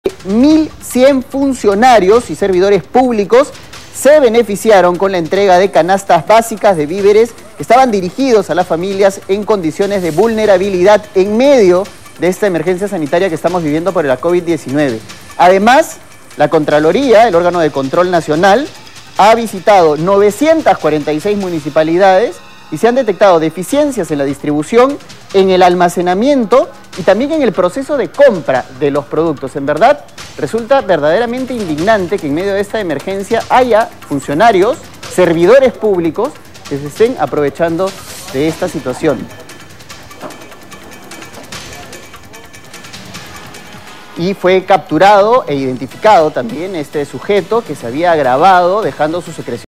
Contraloría: más de mil funcionarios y servidores públicos recibieron canastas de víveres
El contralor general de la República, Nelson Shack, informó que la Contraloría detectó que más de mil 100 funcionarios y servidores públicos recibieron canastas de víveres dirigidas para familias vulnerables por la emergencia ante el coronavirus.

“A pesar que algunos de estos funcionarios ganan más de 8,000 soles, han recibido canastas de víveres, evidentemente eso es parte del control posterior que vamos a realizar apenas concluya la cuarentena", señaló en la sesión virtual de la Comisión de Fiscalización del Congreso.

Shack explicó que este hallazgo se detectó por medio de la plataforma “Transparencia en el marco de la Emergencia Sanitaria COVID-19”, la cual ha permitido hacer un registro y seguimiento de más de un millón de beneficiados.

La Contraloría visitó 946 municipalidades y detectó deficiencias en distribución, almacenamiento, procesos de adquisición y distribución en los productos recibidos para las familias vulnerables.